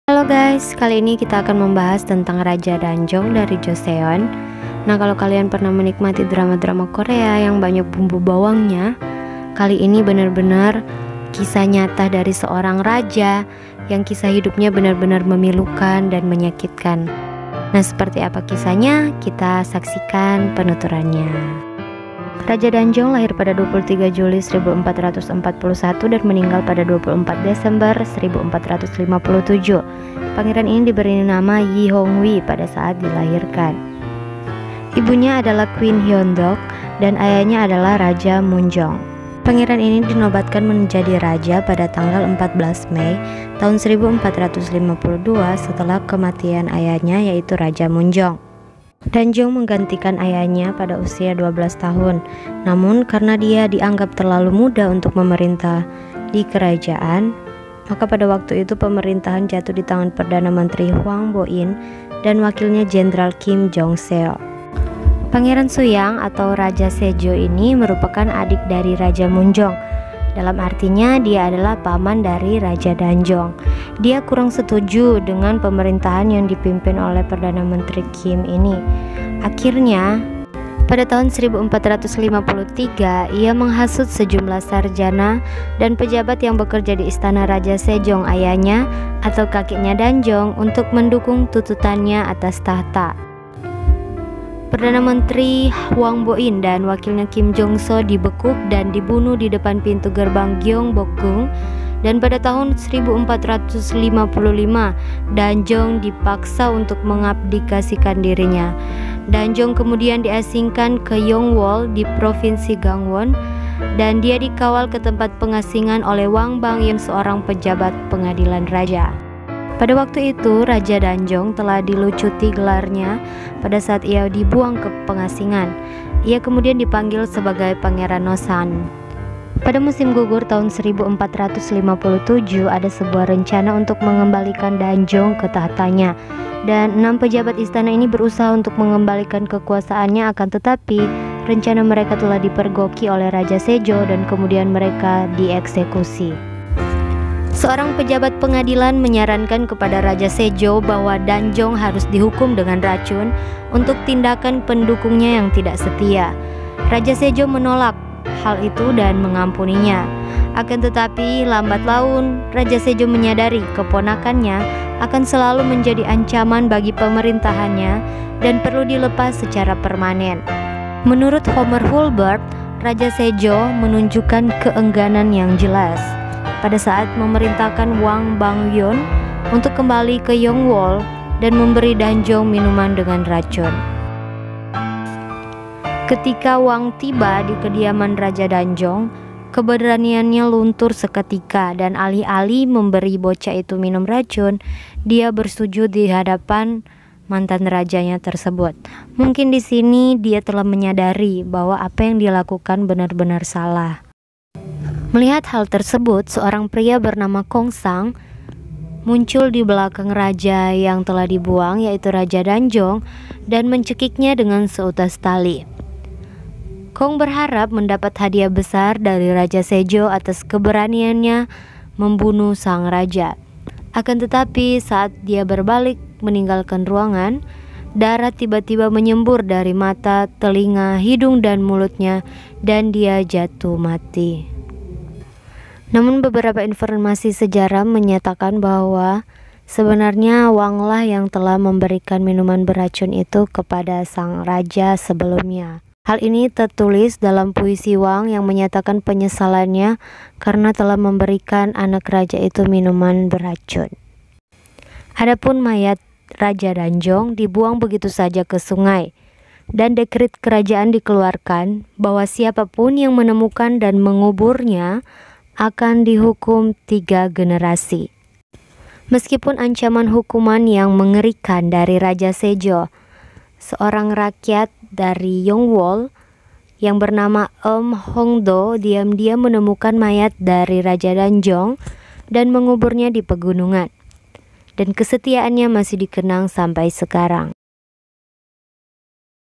KISAH RAJA DANJONG RAJA KE 6 DINASTI JOSEON DIBUNUH SANG PAMAN😭
Hallo guys、 makasi mau mampir lagi ke channel  ini。  ini adalah sepenggal  kisah tentang  intrik Kerajaan  Korea
demi tahta saudara bahkan  keponakan sendiri tak dianggap
miris nggak?
aku、 sedihhh skali  guys

yuk qt simak videonya

btw、 kalo ad koment atau saran、silahkan ya  di bawah

terimakasih😍